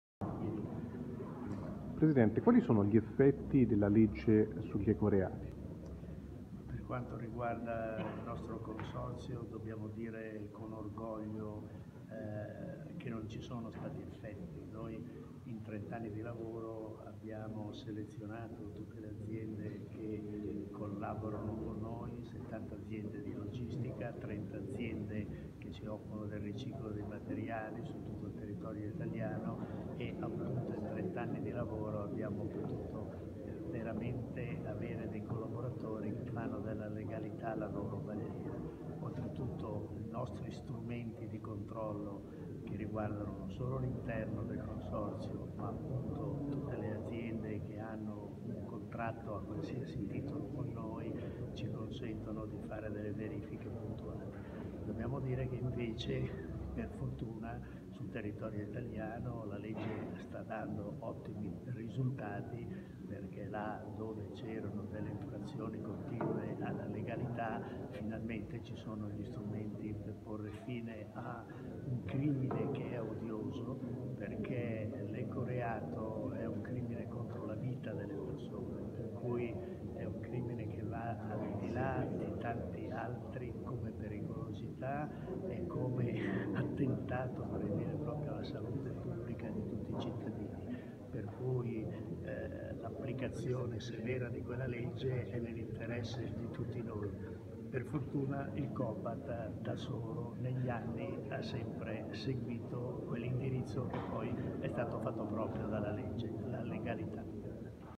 Ambiente, Morandi (Cobat): "Legge ecoreati, ottimi risultati in Italia"
L'intervista a margine del corso di formazione 'Imprese, ambiente e legalita'' dedicato alla legge sugli ecoreati del 2015.